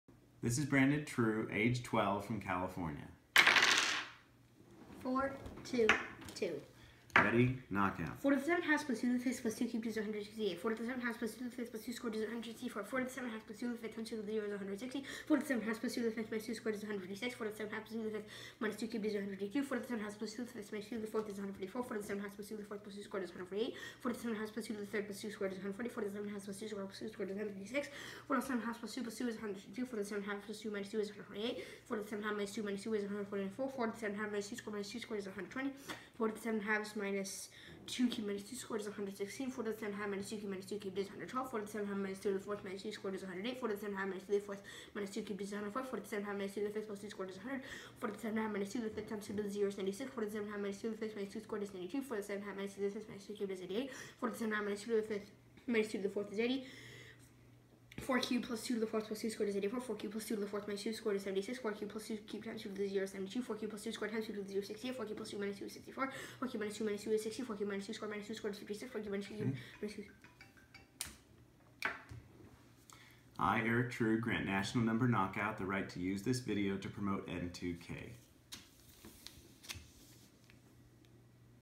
N2K 2016 Brandon True CA
Brandon knocked off 29 out of 36 on the board for a score 3248